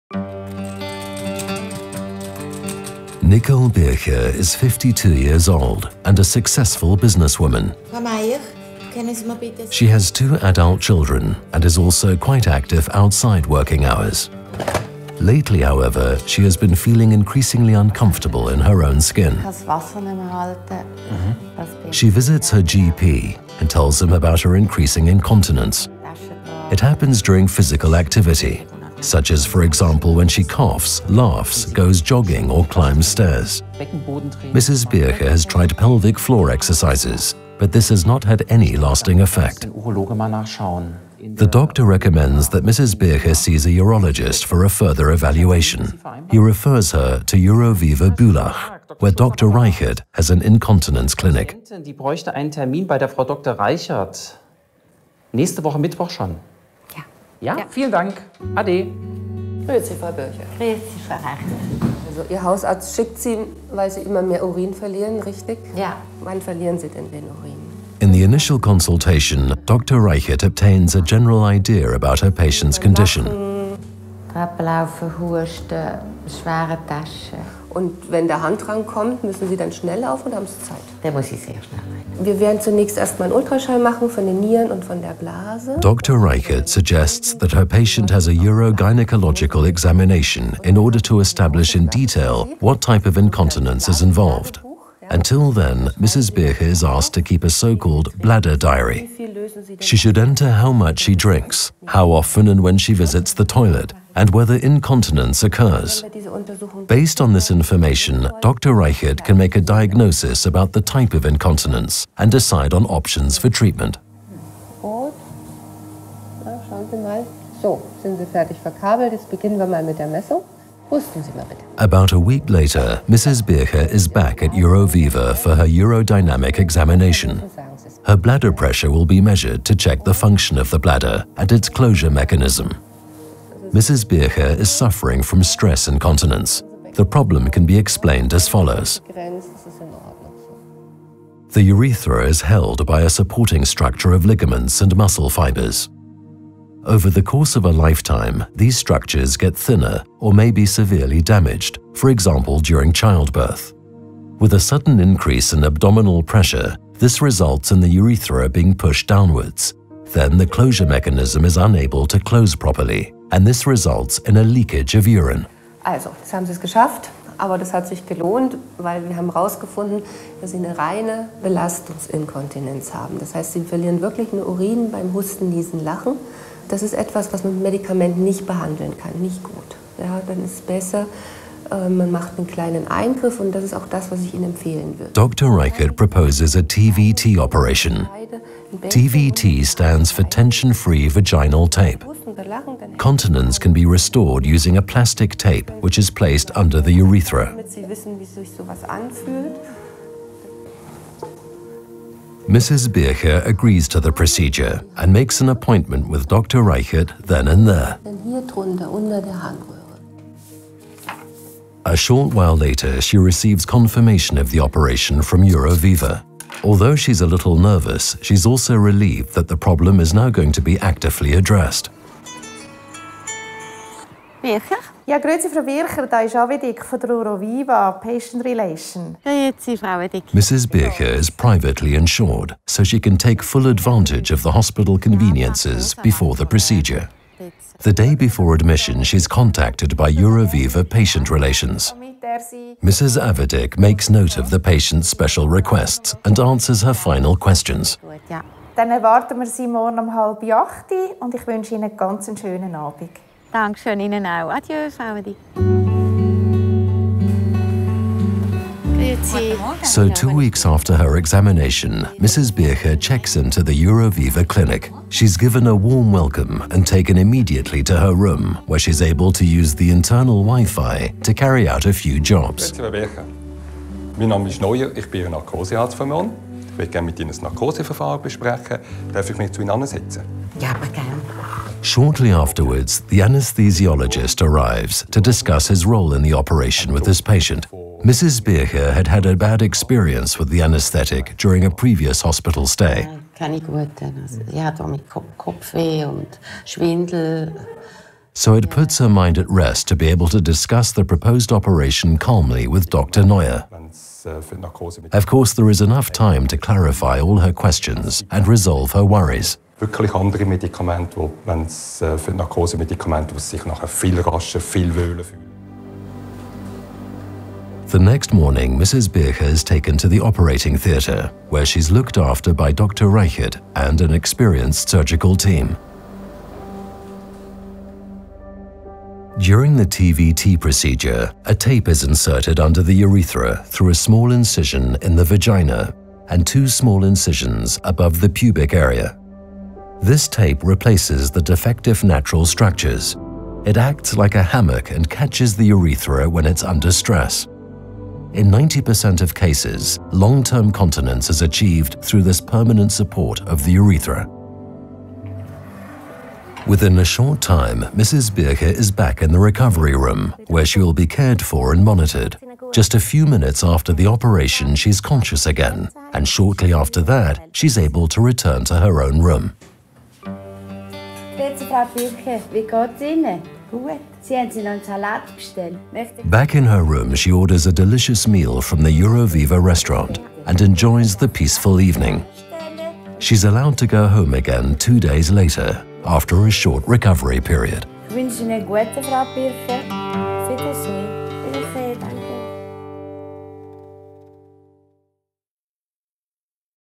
Uroviva - TVT Tape in Women
The 1-cm-wide transvaginal tape (TVT) is placed under the urethra via a small incision in the vagina and two small stab incisions in the groin. The urethra is supported like in a hammock and can no longer fall when it is placed under pressure (e.g. by sneezing, coughing, or jumping). The tape replaces the defective ligament structures; urinary continence is physiologically restored. The success rate of this operation is 84%, which is particularly high in medicine. The tape is made of polypropylene, a plastic which is very well tolerated by the body. The tape is surrounded by the body's own tissue.

Uroviva offers patients and referring physicians from all over the world the full range of urological treatments, using the latest technology.